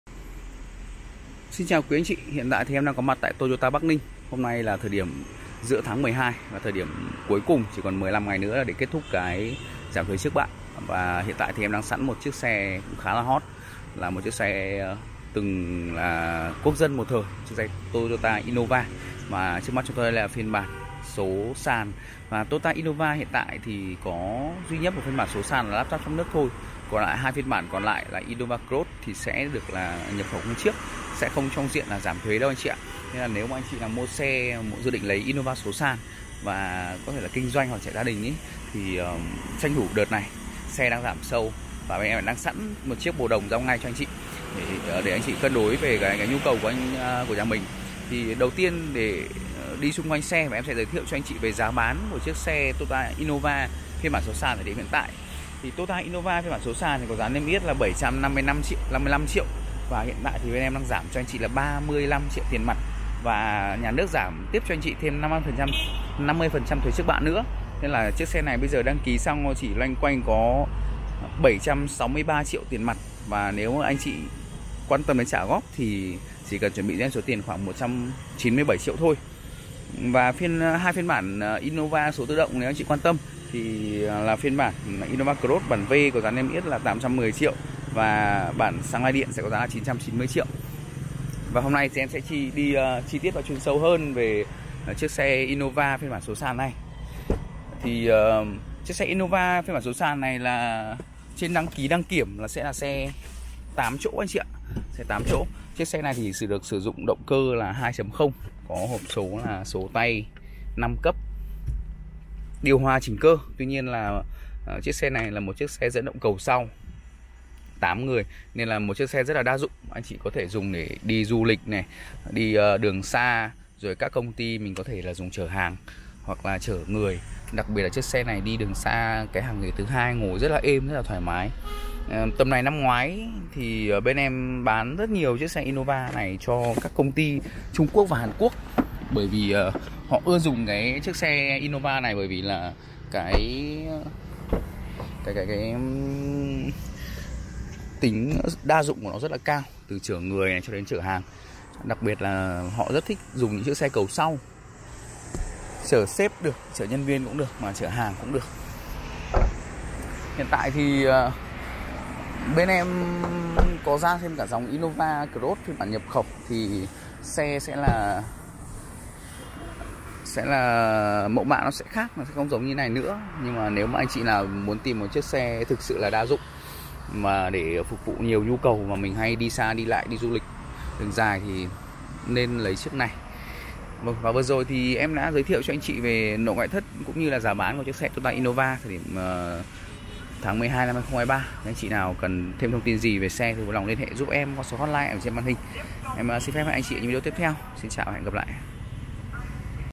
Giá Xe Toyota Innova Số Sàn Màu Đồng Tháng 12/2023 | Tổng Lăn Bánh, Trả Góp Tại Toyota Bắc Ninh
+ Hotline Tư Vấn: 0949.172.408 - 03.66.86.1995